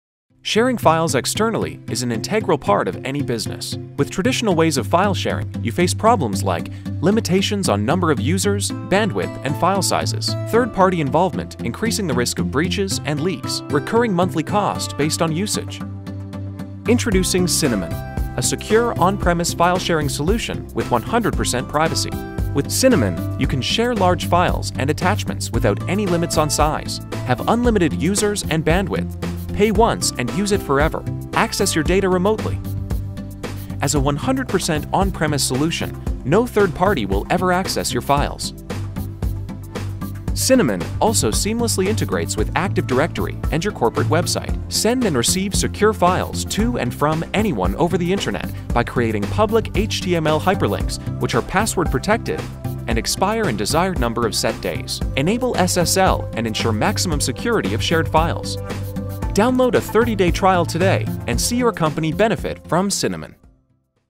Share Files Privately and Securely with SynaMan
Learn how to share files between friends and business colleagues without any privacy concerns. SynaMan is a 100% on-premises file sharing solution with no 3rd party involvement. There are no limits on file size or bandwidth.